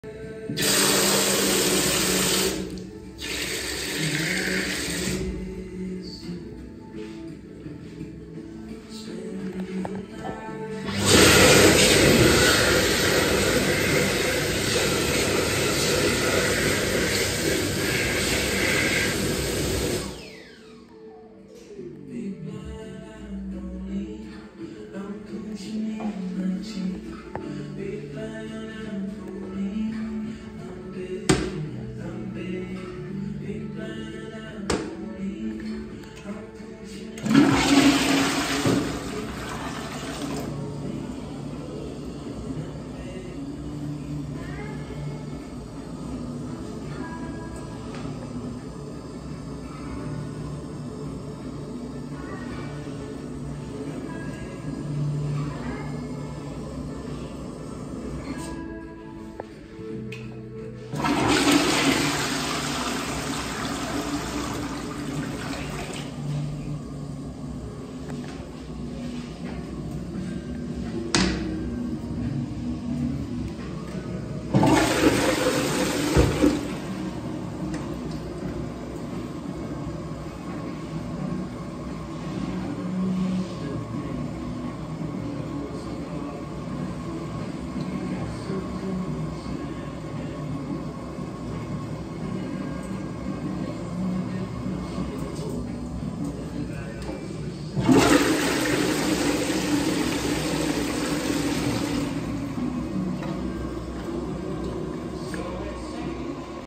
Parents Room Toilet at Grand Central Toowoomba
I knew there would be a trophy in here. There are 2 toilets. A regular Caroma and a Children's Caroma. The Children's Toilet has a more strong flush.